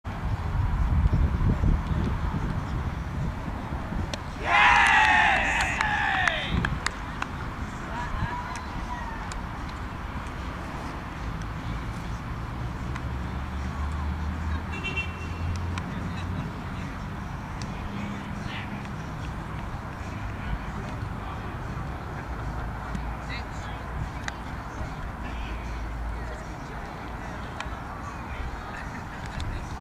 Rowner II v Portsmouth & Southsea
Steve Alford bowls one out in a tight run chase against Portsmouth & Southsea on 19th July 2008 (sorry about the sun...)